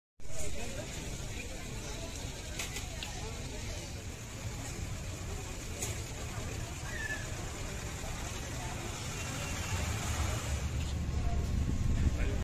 Antojitos y juegos mecánicos se instalan previo a la fiesta de Nuestra Señora del Carmen
En el parque del Carmen, ya se encuentran instalados los puestos de antojitos y juegos mecánicos, previo a la fiesta de Nuestra Señora del Carmen que se conmemora mañana 16 de julio